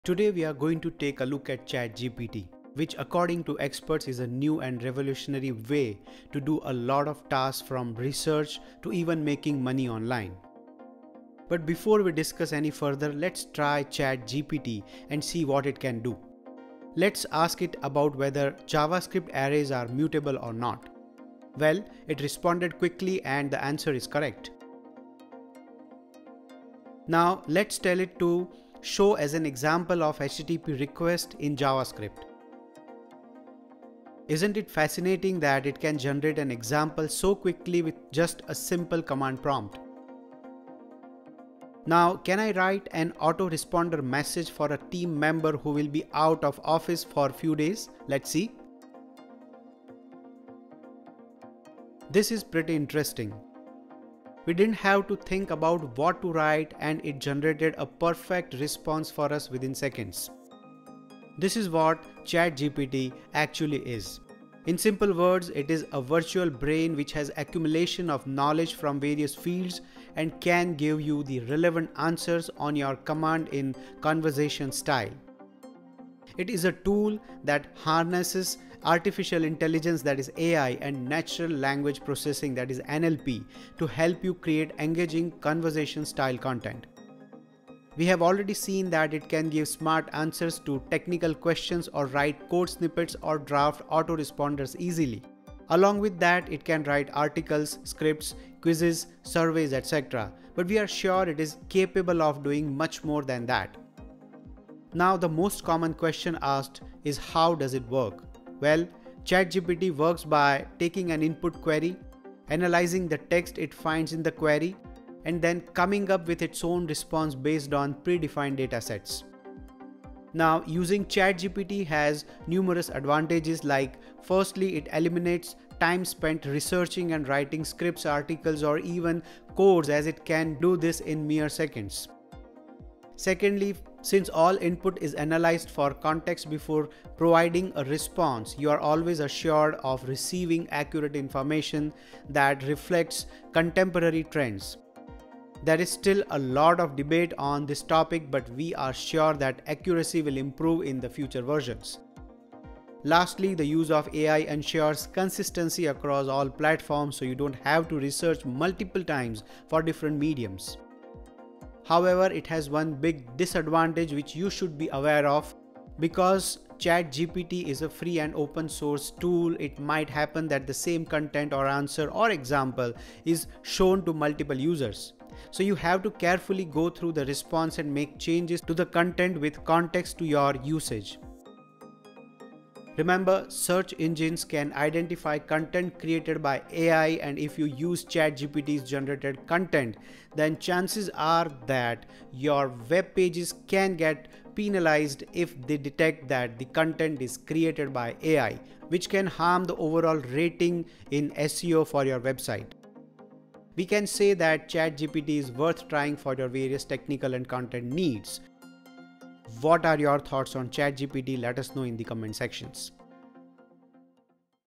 What is chatgpt? | How does ChatGPT work? | How can you use it? | ChatGPT explained
Since its launch in November 2022, it has quickly become one of the most powerful tools available for marketers who want access to high-quality conversational style content quickly and easily.

Let's find out more about ChatGPT in this informative video.